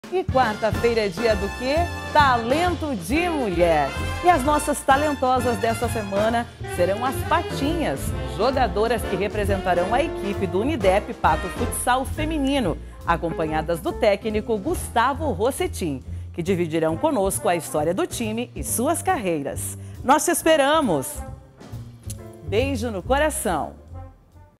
Prévia do Talento de Mulher: com as Patinhas
E as nossas talentosas desta semana serão as Patinhas, jogadoras que representarão a equipe do UNIDEP Pato Futsal Feminino, acompanhadas do Técnico, Gustavo Rossetim, que dividirão conosco a história do time e suas carreiras. Nós te esperamos, beijo no coração.